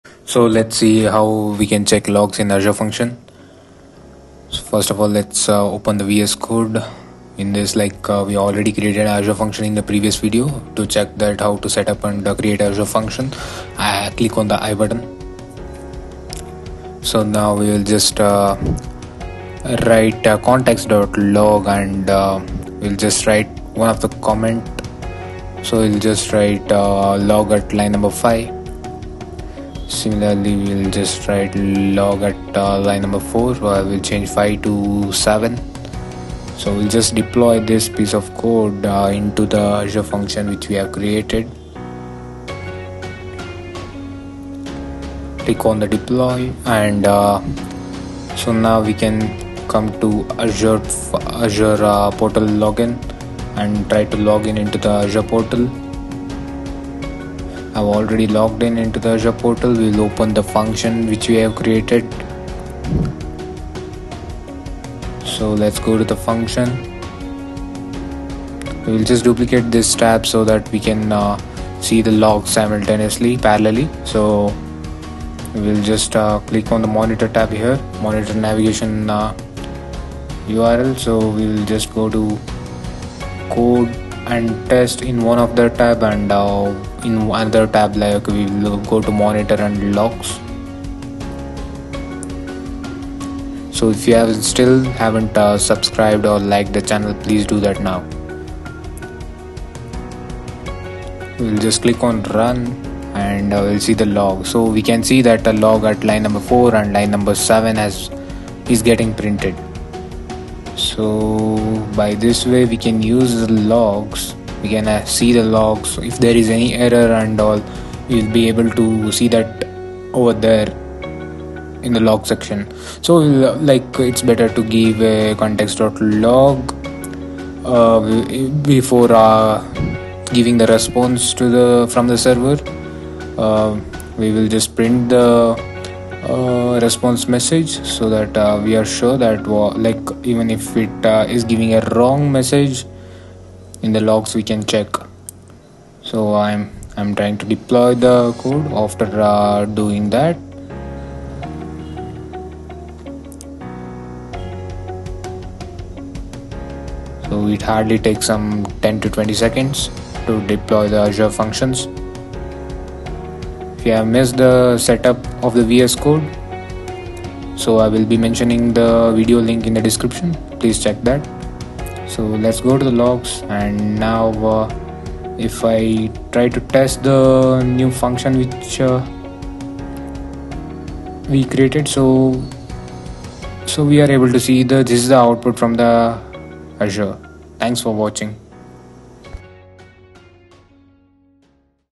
How to check and Implement Azure Function logs - 2022
Check Out

Please free to comment down the questions if you have any.